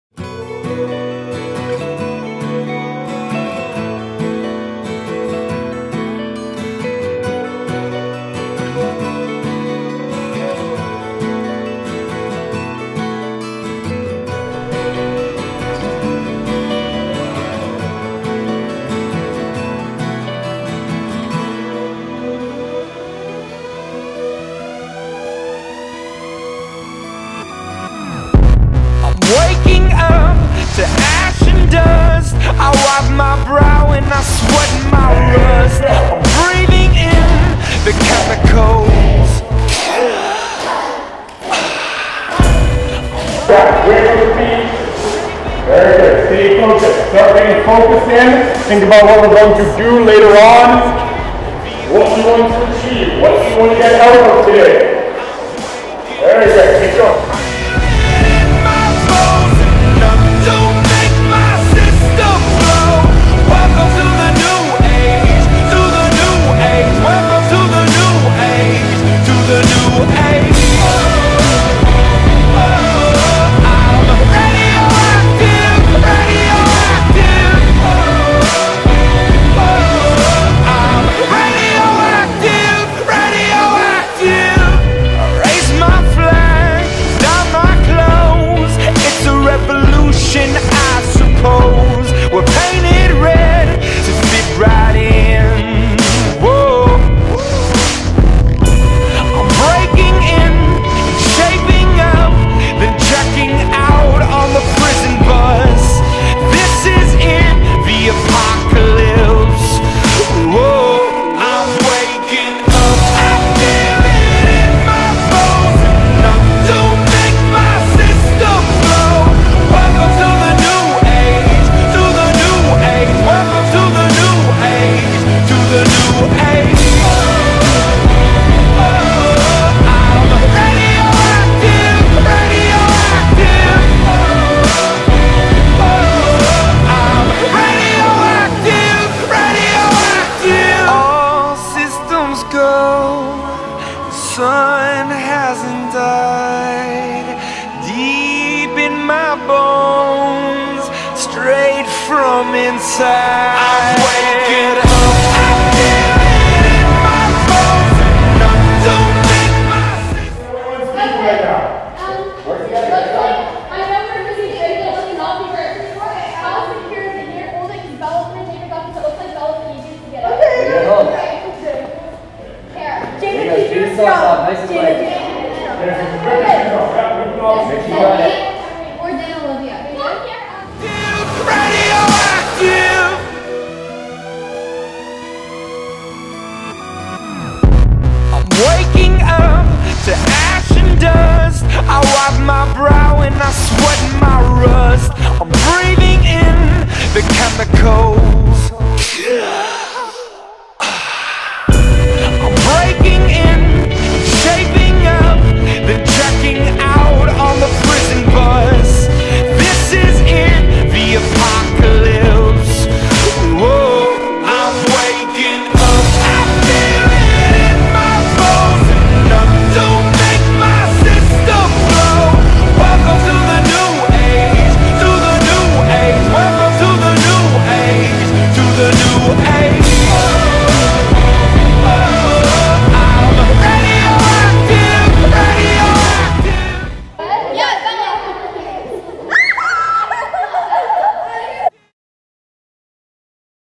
RSF Young Warriors - Richmond Girls Soccer Association
Thank you so much to our partners, Richmond Girls Soccer Association, for giving us the opportunity to work with your athletes.

Here is a sample of what we do!

The music in this video is not owned by Russel Sean Fitness or Richmond Girls Soccer Association.